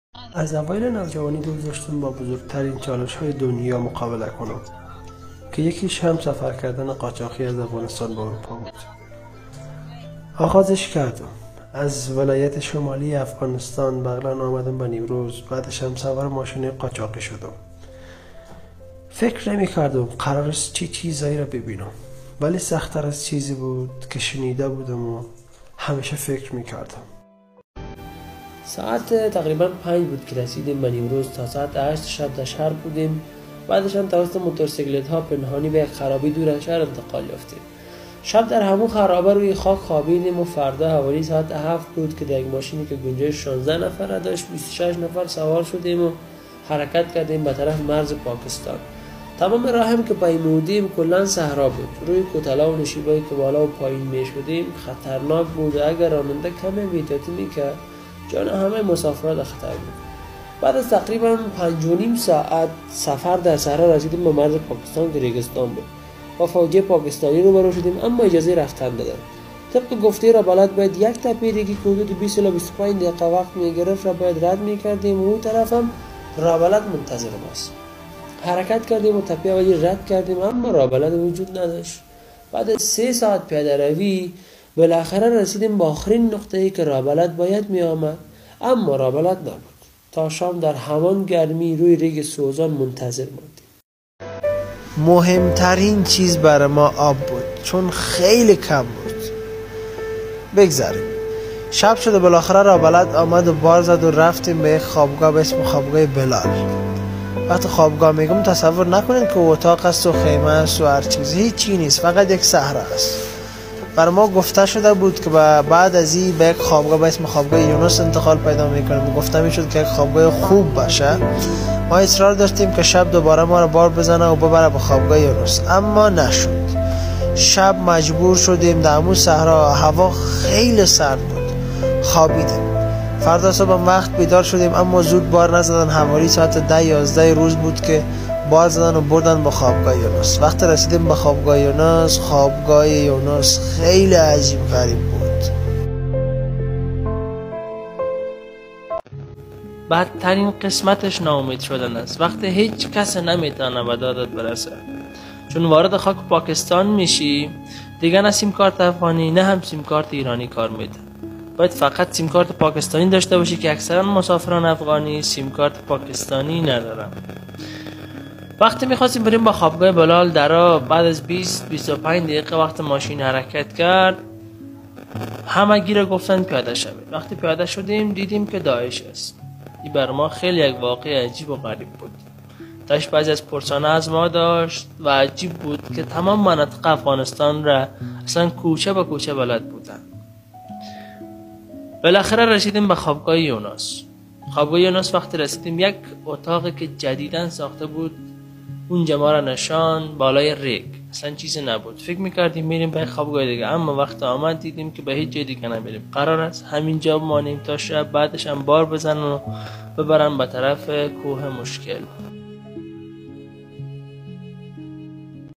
از شمال افغانستان تا قلب اروپا مستند قاچاق (قسمت اول) to heart of Europe from north of Afghanistan
from north Afghanistan to heart of Europe smuggling human and poor afghan people who play Thier lives to bring Thier self to Europe
مستند راه قاچاق از شمال افغانستان تا قلب اروپا افغانانی که با جان خود بازی میکنند تا جان خود را به اروپا برسانند